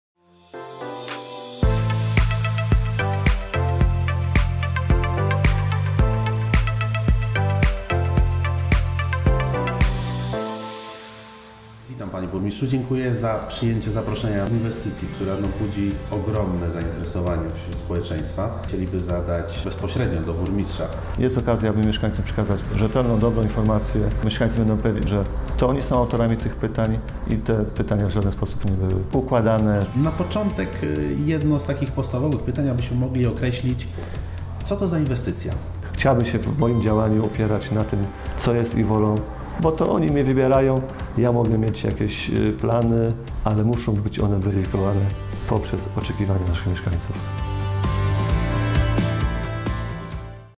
Zapowiedź rozmowy z Burmistrzem Pelplina Mirosławem Chyłą
Zapowiedź rozmowy z Burmistrzem Pelplina Mirosławem Chyłą nt. planowanej inwestycji na terenie gminy Pelplin.